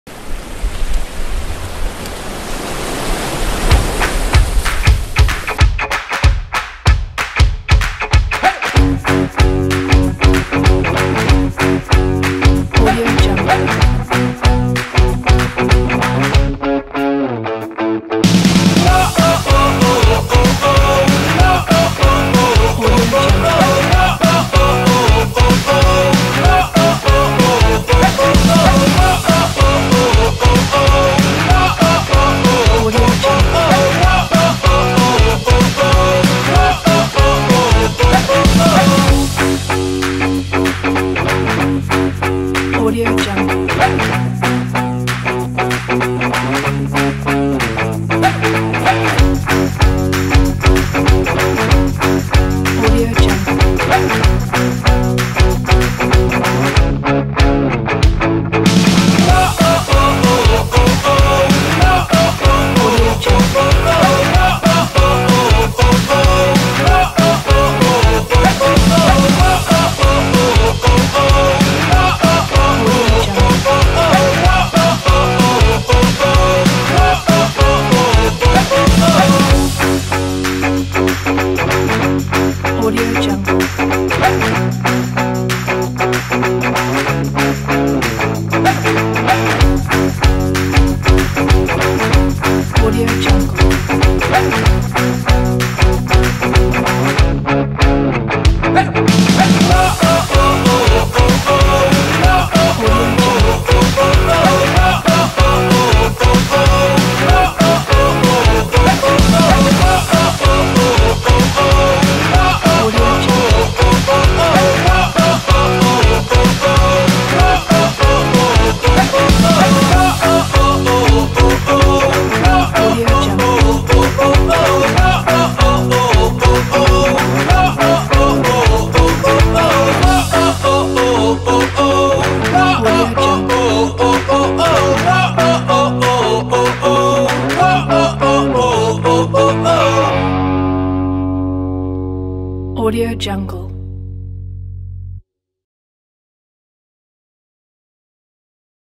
This is what January sailing looks like on Mauritius
Can you see yourself sailing like this in winter?